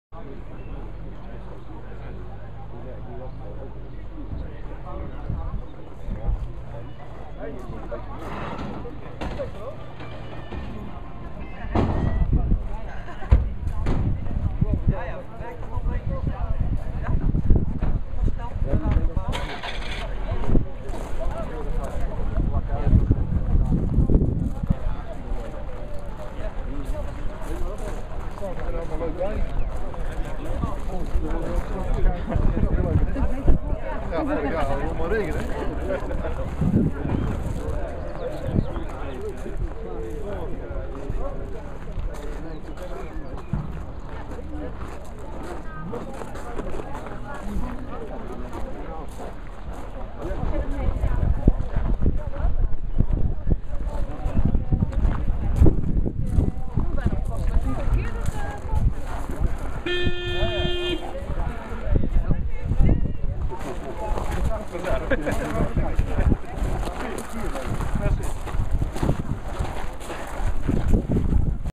Tweede Figaro Tourtocht 2 juni 2013, deel 1
Tweede Figaro Tourtocht 2 juni 2013 van Figaro Team Holland, verzamelen bij De Gallery in Brummen.
Second Figaro Tour june 2 2013 by Figaro Team Holland, Meet at the Gallery in Brummen.